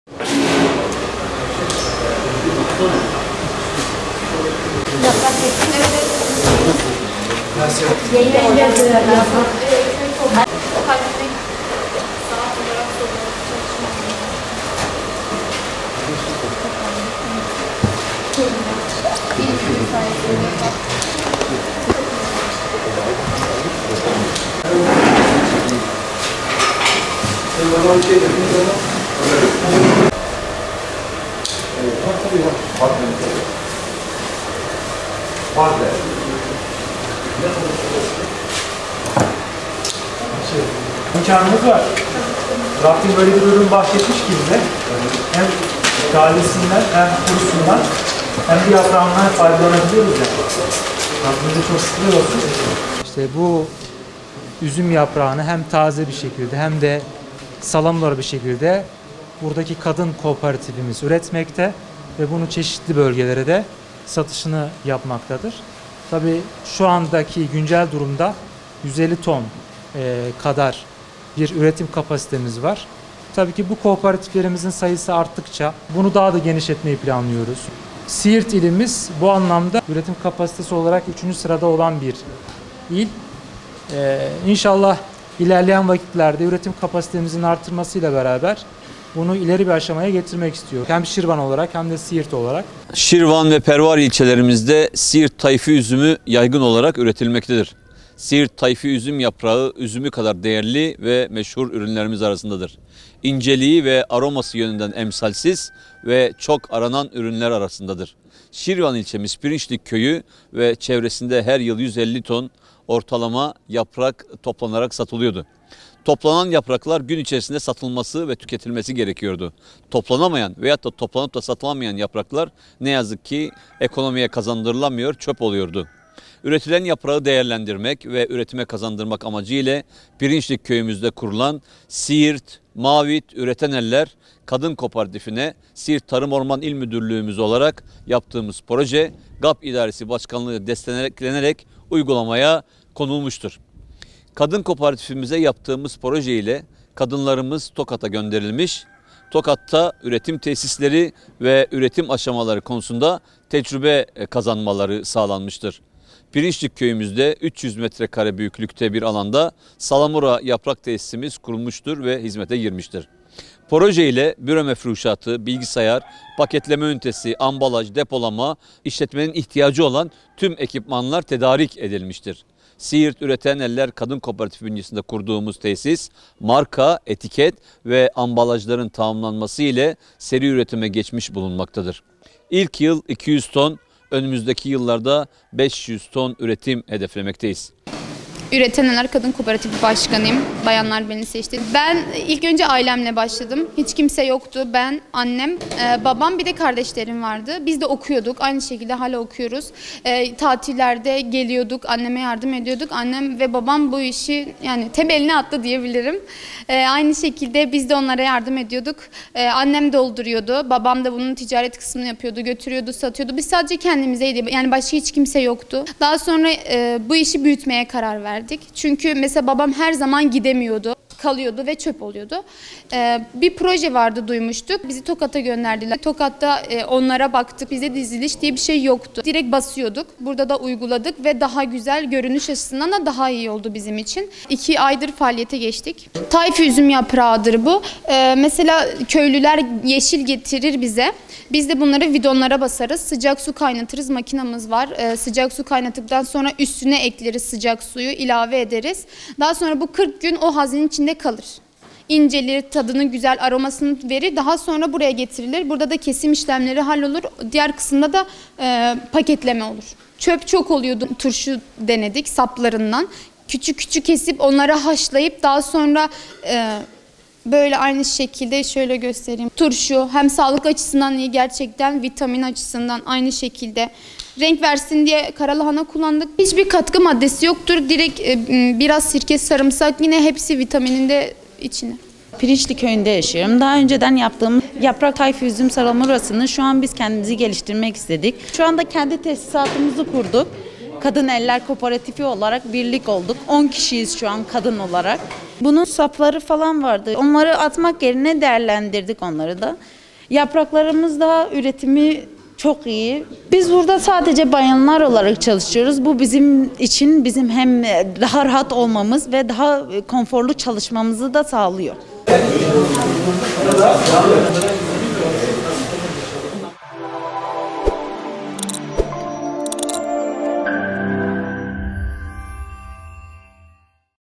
Asma yaprağı köylülerin geçim kaynağı oldu
Asma yaprağının önemli bir gelir kaynağı haline geldiği Siirt'in Şirvan ilçesinde faaliyete geçen 'Salamura Yaprak İşleme Tesisi' köy kadınlarına gelir kapısı oldu.

Değerli İzleyicilerimiz;